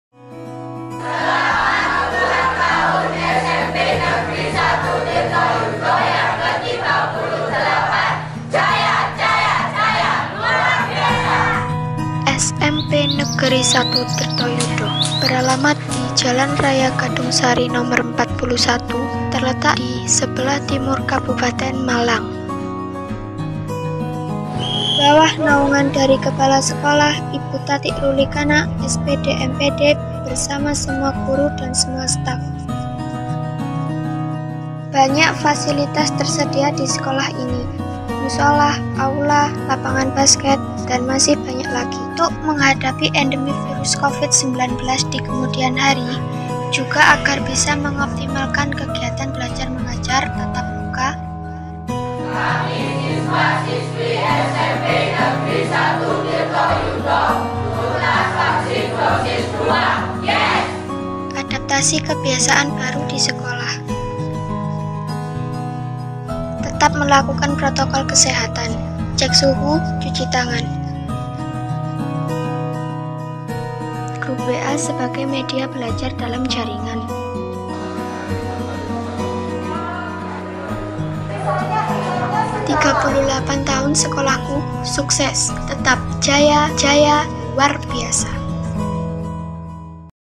Profile Sekolah SMP Negeri 1 Tirtoyudo_Hari jadi yang ke 38 Tahun_tuntas vaksin_Jaya Luar Biasa
Profile sekolah dalam rangka hari jadi yang ke 38 tahun SMP Negeri 1 Tirtoyudo, karya siswi kelas 8D